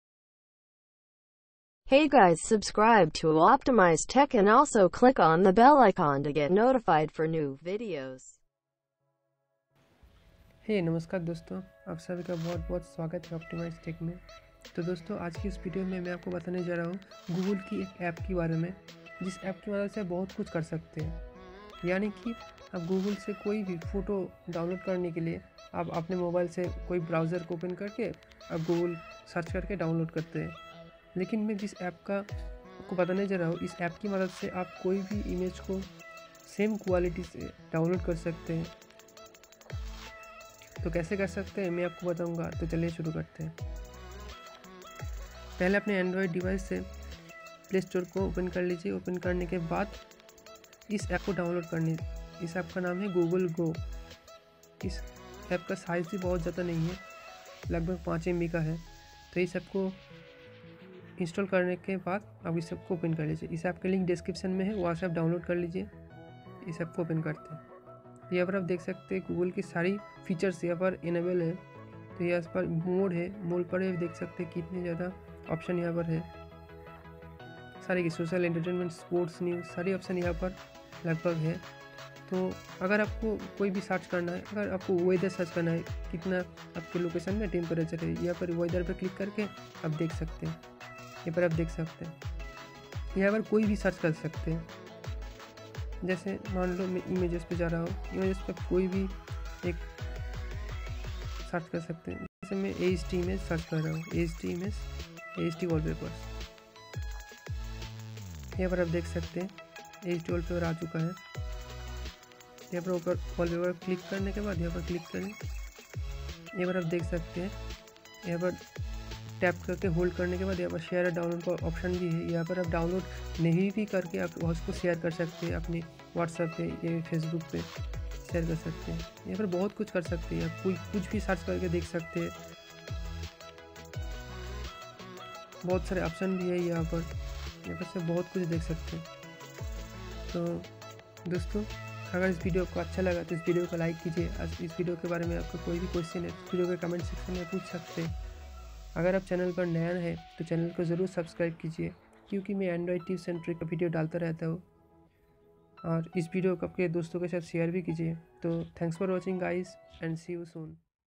All in One App By Google || Google Go
All in one app launch by Google.Size only 5 mb.
Best Google App